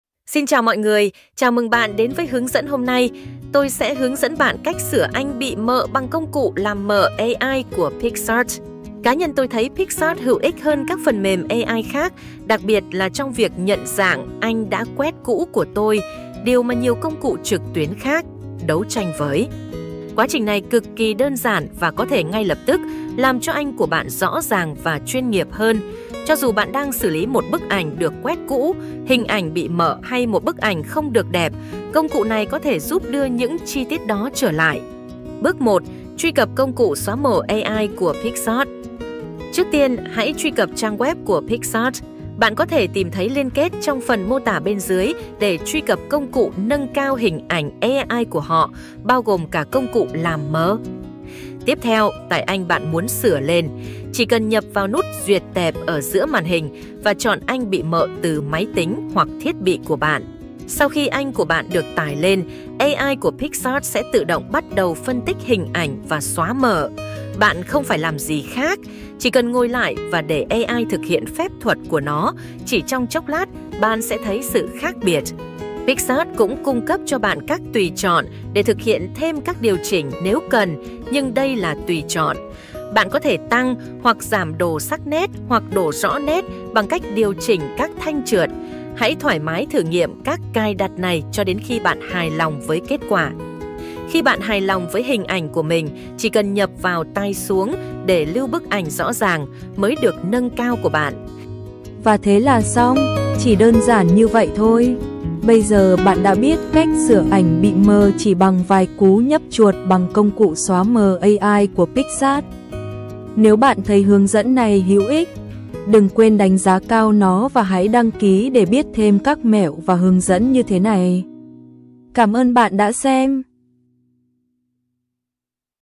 Cách sửa ảnh bị mờ bằng công cụ xóa mờ AI miễn phí
"Xin chào mọi người, chào mừng bạn đến với hướng dẫn hôm nay! Tôi sẽ hướng dẫn bạn cách sửa ảnh bị mờ bằng công cụ làm mờ AI của Picsart. Cá nhân tôi thấy Picsart hữu ích hơn các phần mềm AI khác, đặc biệt là trong việc nhận dạng ảnh đã quét cũ của tôi—điều mà nhiều công cụ trực tuyến khác . đấu tranh với.
Quá trình này cực kỳ đơn giản và có thể ngay lập tức làm cho ảnh của bạn rõ ràng và chuyên nghiệp hơn. Cho dù bạn đang xử lý một bức ảnh được quét cũ, hình ảnh bị mờ hay một bức ảnh không được đẹp, công cụ này có thể giúp đưa những chi tiết đó trở lại."
Bước 1 – Truy cập Công cụ xóa mờ AI của Picsart:
"Trước tiên, hãy truy cập trang web của Picsart. Bạn có thể tìm thấy liên kết trong phần mô tả bên dưới để truy cập công cụ nâng cao hình ảnh AI của họ, bao gồm cả công cụ làm mờ."
"Tiếp theo, tải ảnh bạn muốn sửa lên. Chỉ cần nhấp vào nút 'Duyệt tệp' ở giữa màn hình và chọn ảnh bị mờ từ máy tính hoặc thiết bị của bạn."
"Sau khi ảnh của bạn được tải lên, AI của Picsart sẽ tự động bắt đầu phân tích hình ảnh và xóa mờ. Bạn không phải làm gì khác! Chỉ cần ngồi lại và để AI thực hiện phép thuật của nó. Chỉ trong chốc lát, bạn' sẽ thấy sự khác biệt."
"Picsart cũng cung cấp cho bạn các tùy chọn để thực hiện thêm các điều chỉnh nếu cần, nhưng đây là tùy chọn. Bạn có thể tăng hoặc giảm độ sắc nét hoặc độ rõ nét bằng cách điều chỉnh các thanh trượt. Hãy thoải mái thử nghiệm các cài đặt này cho đến khi bạn hài lòng với kết quả."
"Khi bạn hài lòng với hình ảnh của mình, chỉ cần nhấp vào 'Tải xuống' để lưu bức ảnh rõ ràng, mới được nâng cao của bạn."

"Hello everyone, welcome to today’s tutorial! I’ll be showing you how to fix blurry photos using Picsart’s AI unblurring tool. I personally found Picsart more helpful than other AI software, especially for recognizing my old scanned photos—something many other online tools struggle with.
The process is super simple and can instantly make your photos clearer and more professional looking. Whether you’re dealing with an old scanned photos, blurry image or a shot that didn’t turn out quite right, this tool can help bring those details back."
Step 1 – Accessing Picsart's AI Unblurring Tool:
"Next, upload the photo you want to fix. Simply click on the 'Browse file' button in the center of the screen, and choose the blurry photo from your computer or device."
"Once your photo is uploaded, Picsart’s AI will automatically start analyzing the image and removing the blur. You don’t have to do anything else! Just sit back and let the AI work its magic. In a few moments, you’ll see the difference."
"Picsart also gives you options to make further adjustments if needed, but this is optional. You can increase or decrease the sharpness or clarity by adjusting the sliders. Feel free to play around with these settings until you’re happy with the result."
"When you’re satisfied with how your image looks, just click 'Download' to save your newly enhanced, clear photo."

"And that’s it! It’s as simple as that. Now you know how to fix blurry photos in just a few clicks using Picsart’s AI unblurring tool.

"Và thế là xong! Chỉ đơn giản như vậy thôi. Bây giờ bạn đã biết cách sửa ảnh bị mờ chỉ bằng vài cú nhấp chuột bằng công cụ xóa mờ AI của Picsart. Cảm ơn bạn đã xem!"